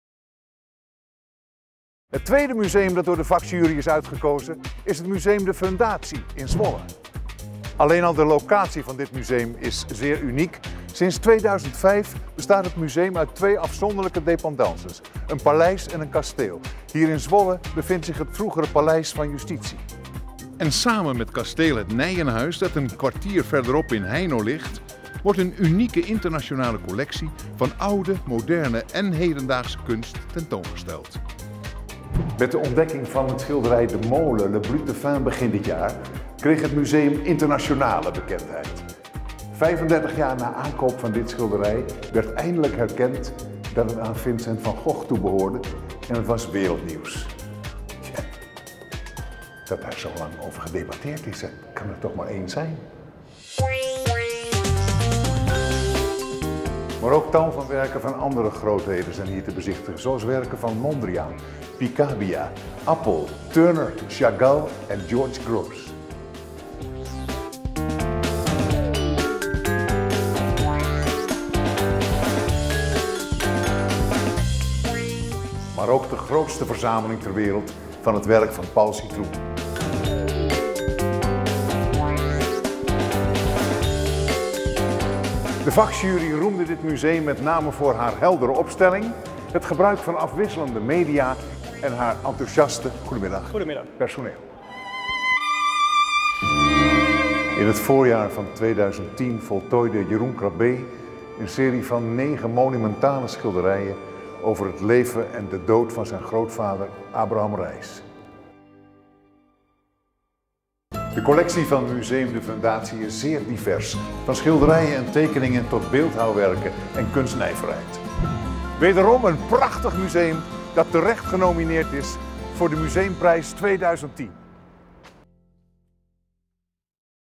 BankGiro Loterij Museumprijs 2010 | Genomineerde: De Fundatie Zwolle
Ron Brandsteder bezoekt Museum de Fundatie in Zwolle, een van de 3 genomineerden voor de BankGiro Loterij Museumprijs 2010. Stem mee op